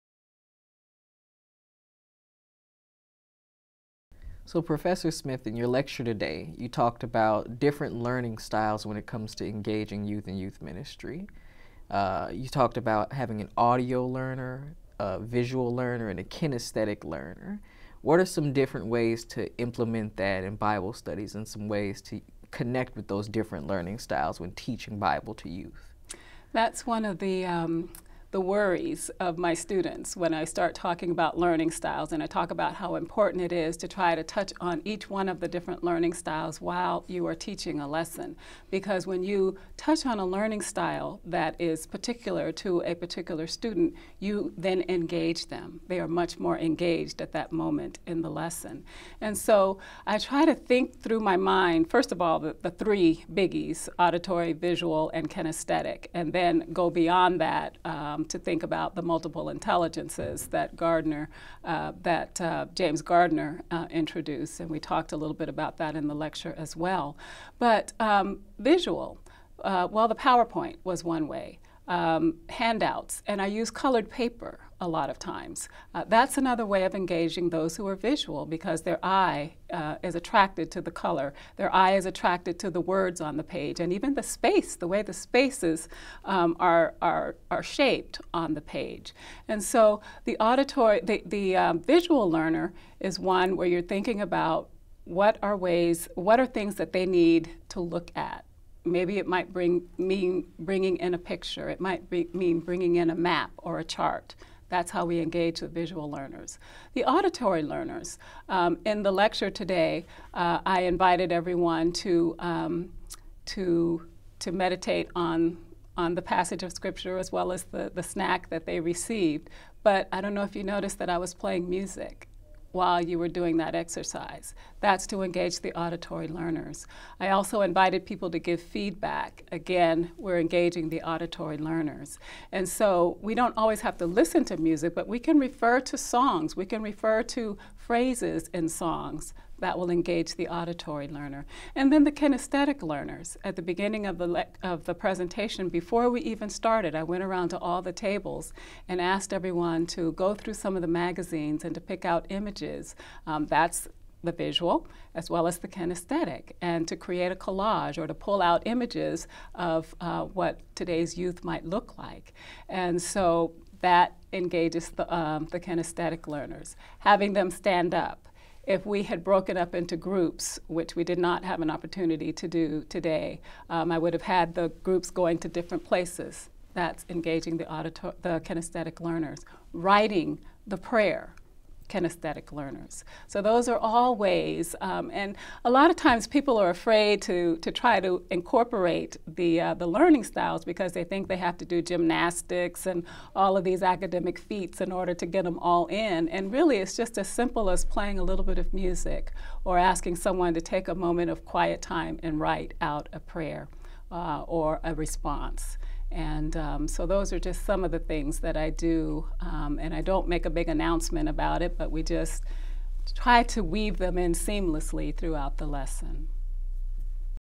Implementing Learning Styles - Rev. Dr. Yolanda Smith
Rev. Dr. Yolanda Smith explains the importance of incorporating different learning styles into lessons, following her YMI lecture, "Exploring the Bible With Contemporary Youth."

Rev. Dr. Yolanda Smith is a research scholar and lecturer in Christian Education at Yale Divinity School and an ordained Baptist minister.